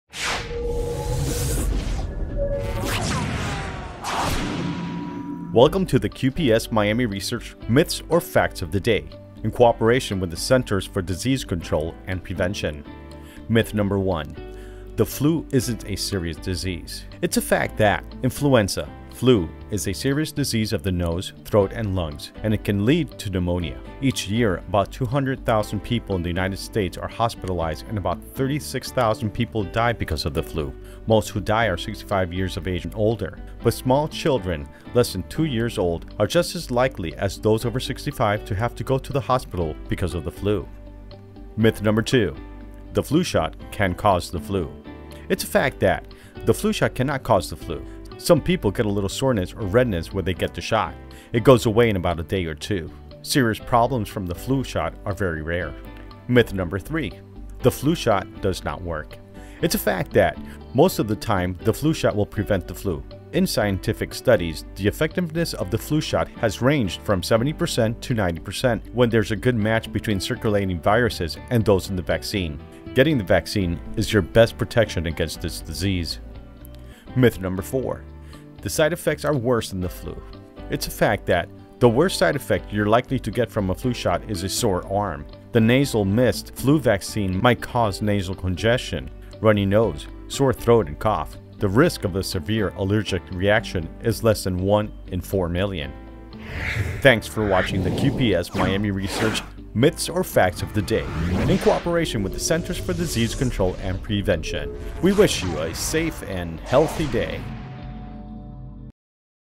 QPS Miami Research Myths or Facts about the Flu and Flu Shots
Welcome to the Q.P.S. Miami Research... "MYTHS OR FACTS of the day", in cooperation with the Centers for Disease Control and Prevention.

MYTH NUMBER ONE
"The flu isn't a serious disease."
IT's A FACT THAT...
Influenza (flu) is a serious disease of the nose, throat, and lungs, and it can lead to pneumonia. Each year about 200,000 people in the U.S. are hospitalized and about 36,000 people die because of the flu. Most who die are 65 years and older. But small children less than 2 years old are as likely as those over 65 to have to go to the hospital because of the flu.

MYTH NUMBER TWO
"The flu shot can cause the flu."
The flu shot cannot cause the flu. Some people get a little soreness or redness where they get the shot. It goes away in a day or two. Serious problems from the flu shot are very rare.

MYTH NUMBER THREE
"The flu shot does not work."
Most of the time the flu shot will prevent the flu. In scientific studies, the effectiveness of the flu shot has ranged from 70% to 90% when there is a good match between circulating viruses and those in the vaccine. Getting the vaccine is your best protection against this disease.

MYTH NUMBER FOUR
"The side effects are worse than the flu'
The worst side effect you're likely to get from a  shot is a sore arm. The nasal mist flu vaccine might cause nasal congestion, runny nose, sore throat and cough. The risk of a severe allergic reaction is less than
1 in 4 million.

... We wish you a safe and healthy day!

This content has been provided by the Center for Disease Control and Prevention.